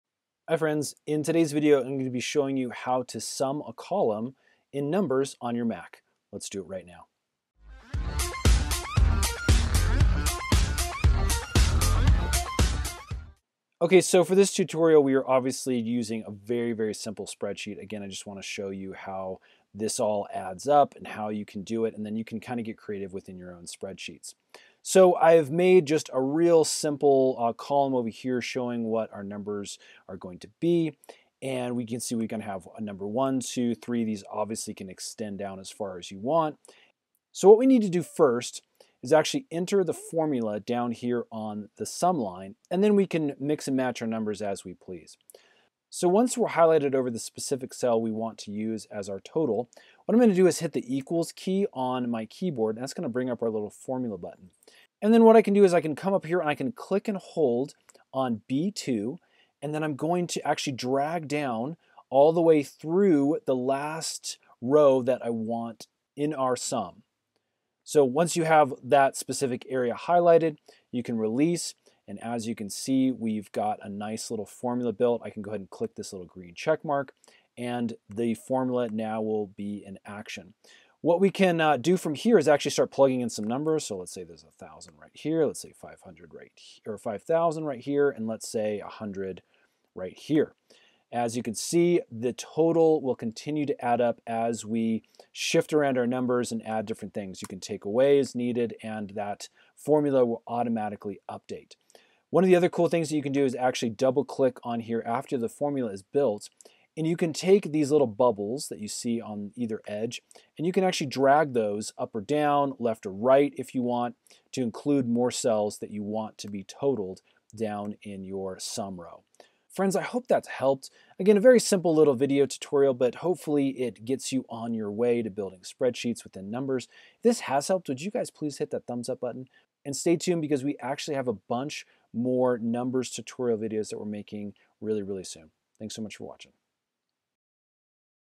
How to Sum a Column or Cells in Apple Numbers
Check out today's special tech deals:

In this video, we show you how to sum columns and cells in Apple Numbers. This is a very simple process that we walk you through step-by-step. We show you the easiest way to create sum functions for columns which can also be applied to rows. By the end of this video you'll be able to use this function within your own Apple Number spreadsheet.